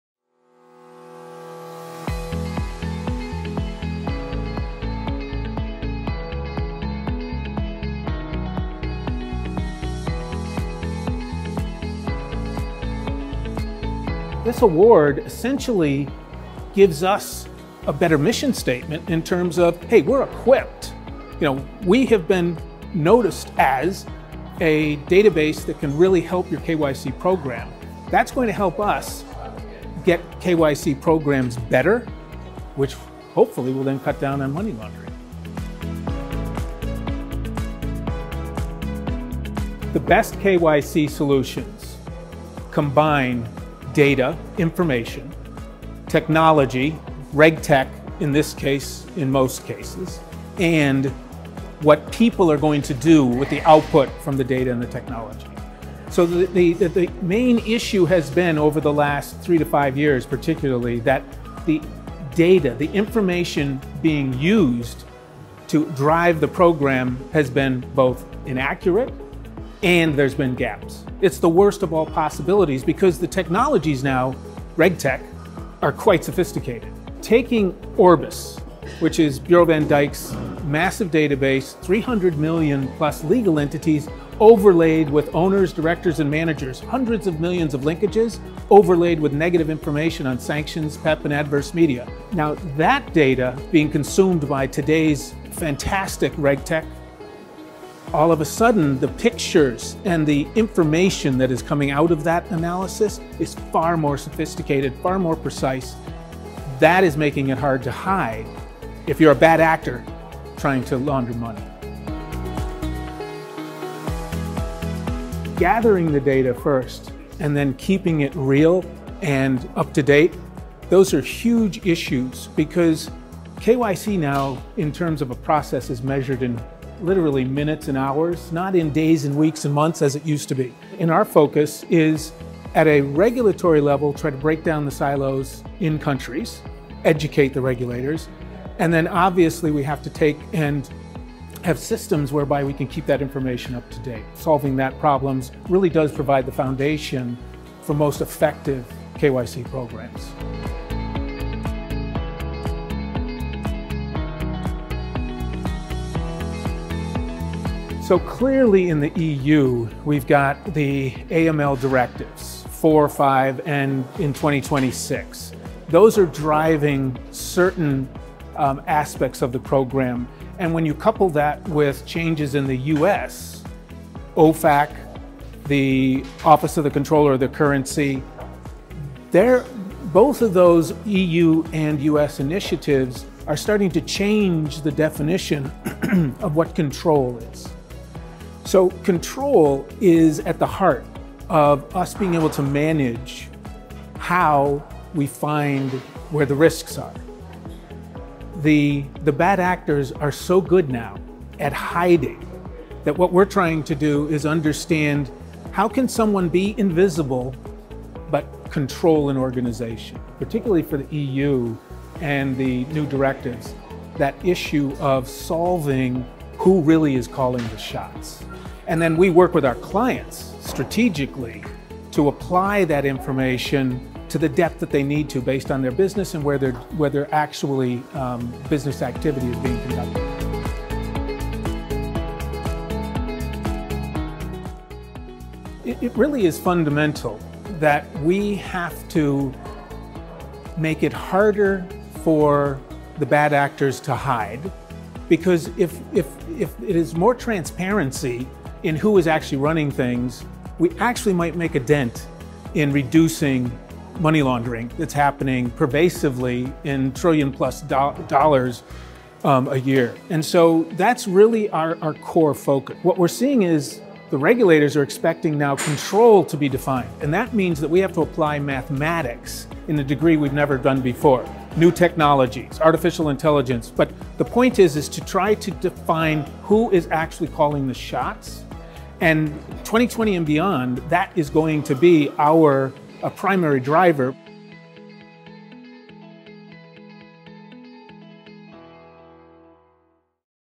Bureau van Dijk wins Best Data Solution for KYC at 2019 RegTech Insight Awards
Bill Hauserman, senior director, compliance solutions at Bureau van Dijk, explains why Bureau van Dijk and Orbis were awarded "Best Data Solution for KYC" at the 2019 RegTech Insight Awards and what lies ahead.

"Control is at the heart of us being able to manage how we find where the risks are. The bad actors are so good now at hiding, that what we're trying to do is understand, how can someone be invisible but control an organization?"